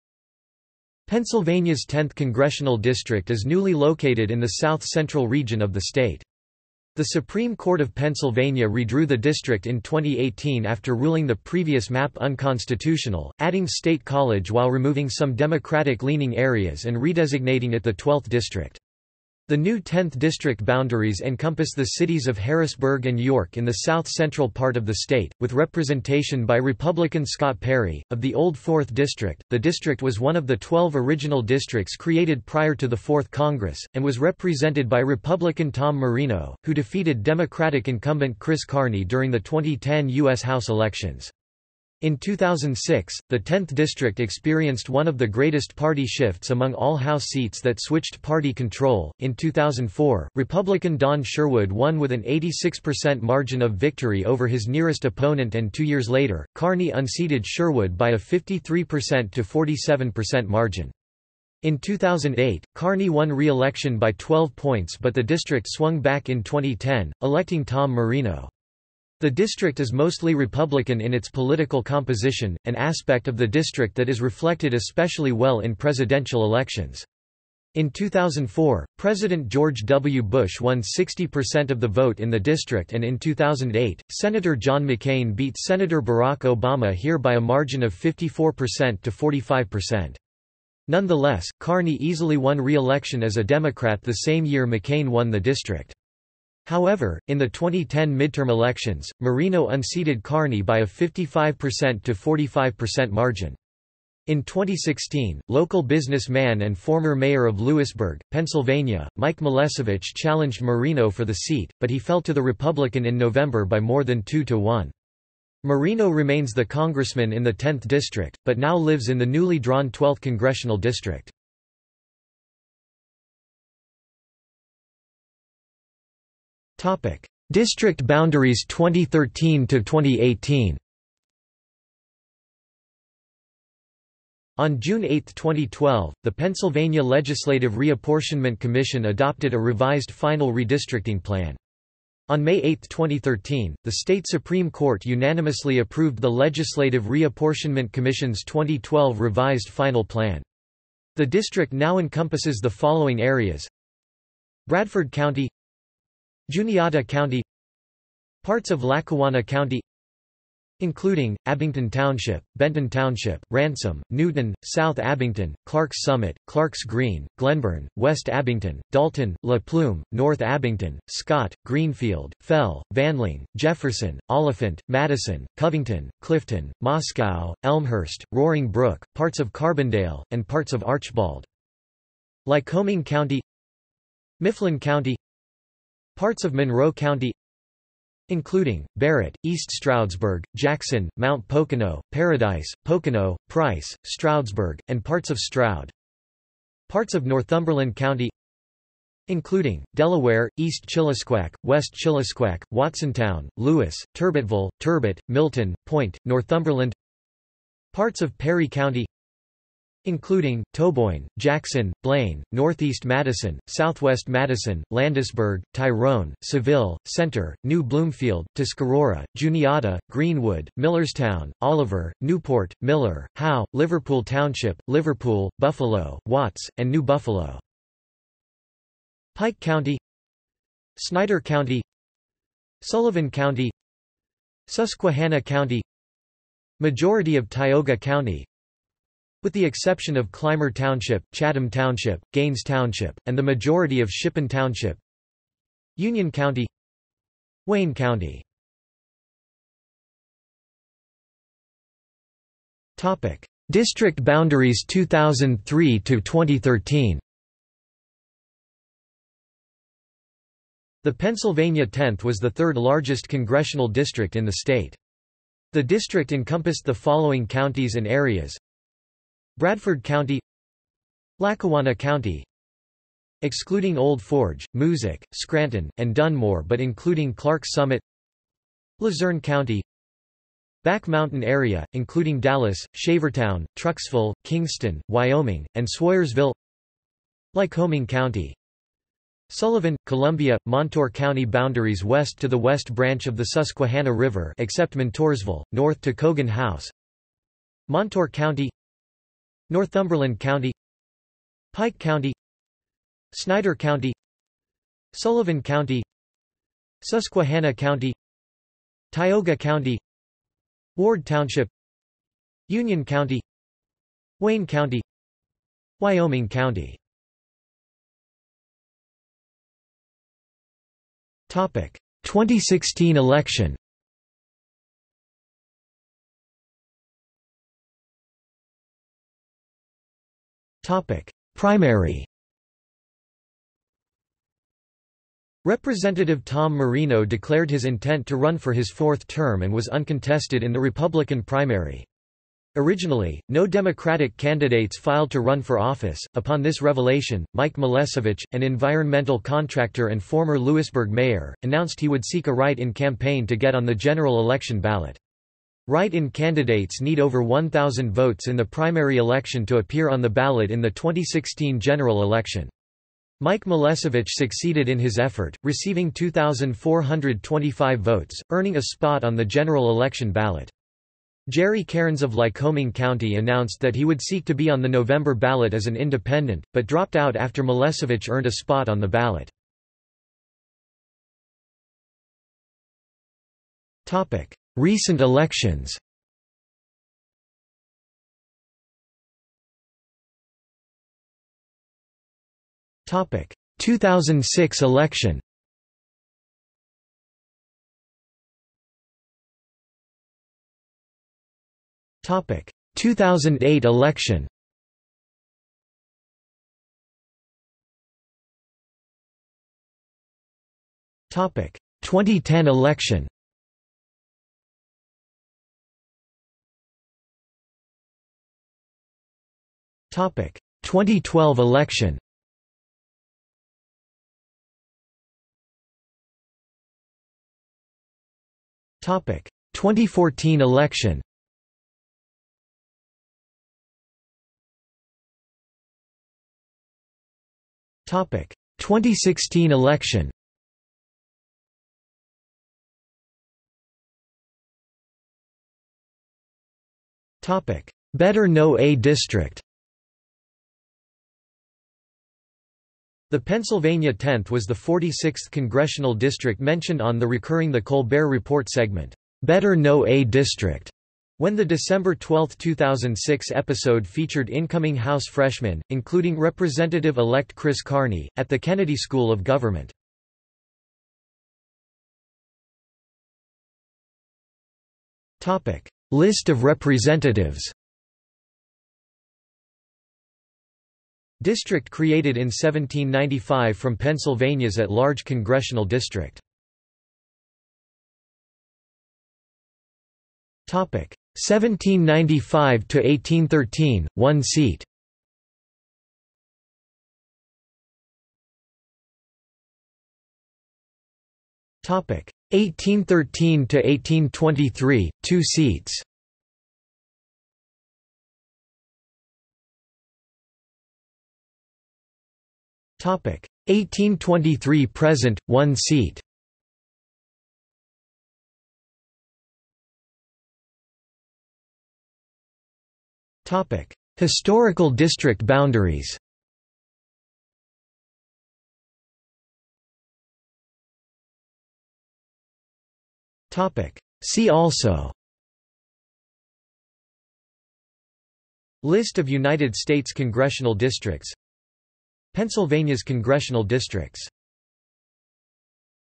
Pennsylvania's 10th congressional district | Wikipedia audio article
This is an audio version of the Wikipedia Article:
Pennsylvania's 10th congressional district

00:02:03 1 District boundaries 2013–2018
00:04:09 2 District boundaries 2003–2013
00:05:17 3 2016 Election
00:05:27 3.1 Primary
00:06:20 4 Recent elections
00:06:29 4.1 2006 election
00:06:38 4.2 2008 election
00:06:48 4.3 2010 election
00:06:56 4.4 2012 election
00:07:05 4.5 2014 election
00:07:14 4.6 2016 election
00:07:23 5 Better Know A District
00:07:51 6 List of representatives
00:08:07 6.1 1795–1813: One seat
00:08:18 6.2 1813–1823: Two seats
00:08:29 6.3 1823–present: One seat
00:08:39 7 Historical district boundaries
00:08:49 8 See also

- Socrates

SUMMARY
Pennsylvania's 10th Congressional District is newly located in the south-central region of the state. The Supreme Court of Pennsylvania redrew the district in 2018 after ruling the previous map unconstitutional, adding State College while removing some Democratic-leaning areas and redesignating it the twelfth district. The new tenth district boundaries encompass the cities of Harrisburg and York in the south-central part of the state, with representation by Republican Scott Perry, of the old fourth district.The district was one of the 12 original districts created prior to the 4th Congress, and was represented by Republican Tom Marino, who defeated Democratic incumbent Chris Carney during the 2010 U.S. House elections. In 2006, the 10th district experienced one of the greatest party shifts among all House seats that switched party control: in 2004, Republican Don Sherwood won with an 86% margin of victory over his nearest opponent and two years later, Carney unseated Sherwood by a 53%–47% margin. In 2008, Carney won reelection by 12 points but the district swung back in 2010, electing Tom Marino. The district is mostly Republican in its political composition, an aspect of the district that is reflected especially well in presidential elections. In 2004, President George W. Bush won 60 percent of the vote in the district and in 2008, Senator John McCain beat Senator Barack Obama here by a margin of 54 percent to 45 percent. Nonetheless, Carney easily won reelection as a Democrat the same year McCain won the district. However, in the 2010 midterm elections, Marino unseated Carney by a 55%–45% margin. In 2016, local business man and former mayor of Lewisburg, Pennsylvania, Mike Molesevich challenged Marino for the seat, but he fell to the Republican in November by more than two to one. Marino remains the congressman in the 10th district, but now lives in the newly drawn 12th Congressional District.